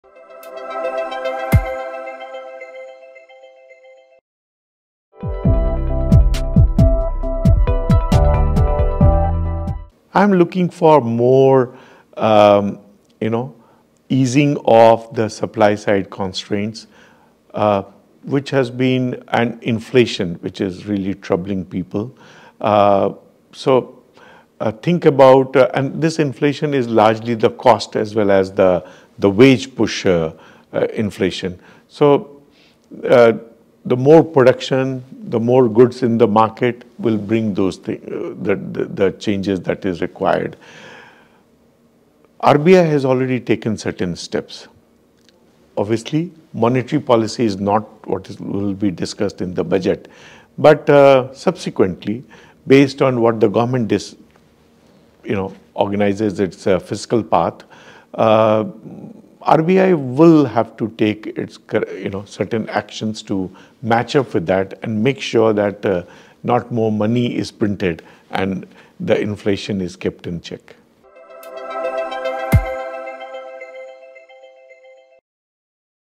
Union Budget 2023: Easy supply constraints to control inflation
Union Budget 2023: Finance Minister Nirmala Sitharaman should look to further ease supply side constraints so that inflation can be kept under check, because it has really been troubling people, says Sanjay Kumar, Partner, Deloitte India.